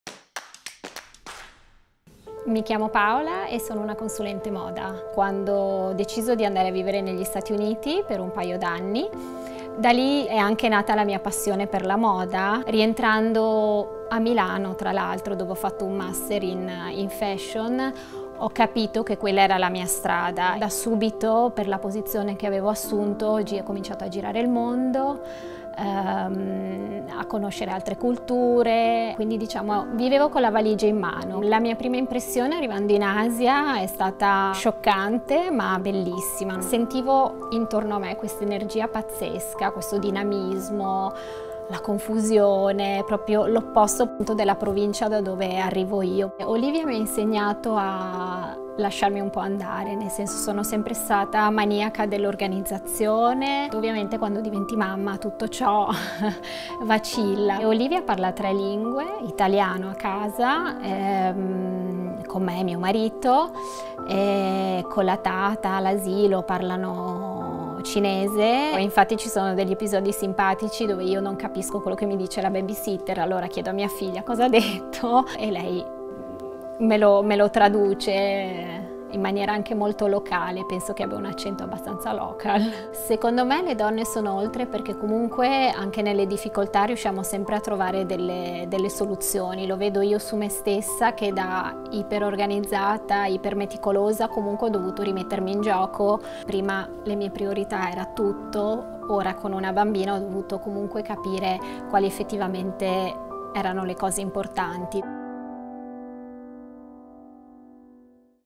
Oltre | Women'stories | Paola Talamini Minotto
Un nuovo format video con appuntamenti settimanali che aprono una finestra sulla vista più bella di tutte: le storie ordinarie di donne straordinarie.

8 protagoniste per 8 momenti di condivisione.

In ogni puntata, sorelle, amiche, moglie e madri si raccontano in Casa Oltre ripercorrendo le strade della propria vita, svelandone i segreti e i progetti futuri. 8 sogni realizzati e qualcuno ancora da scartare, di chi è riuscita ad andare, sentire, vedere e vivere Oltre.

Questa è la storia di Paola Talamini Minotto